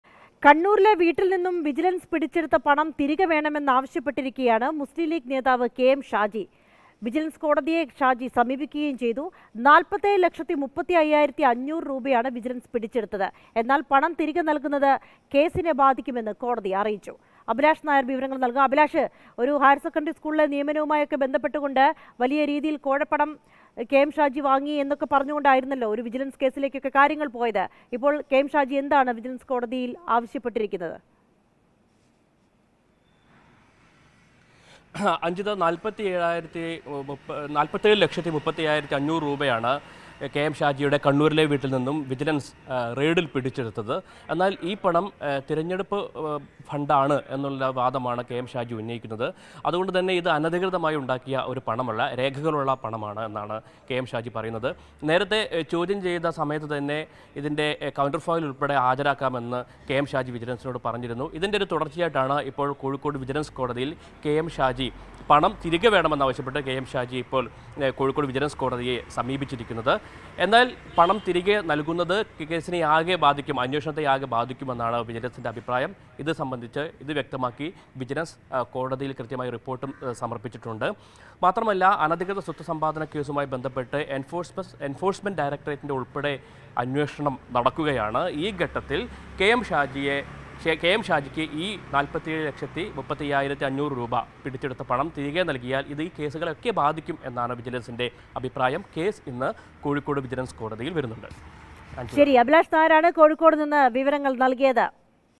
വീട്ടില്‍ നിന്ന് വിജിലന്‍സ് പിടിച്ചെടുത്ത പണം തിരികെ ആവശ്യപ്പെട്ട് കെഎം ഷാജി| Mathrubhumi News
കണ്ണൂരിലെ വീട്ടില്‍ നിന്ന് വിജിലന്‍സ് പിടിച്ചെടുത്ത പണം തിരികെ ആവശ്യപ്പെട്ട് കെഎം ഷാജി വിജിലന്‍സ് കോടതിയെ സമീപിച്ചു. പണം തിരികെ നല്‍കുന്നത് കേസിനെ ബാധിക്കുമെന്ന് കോടതി.

Happy viewing!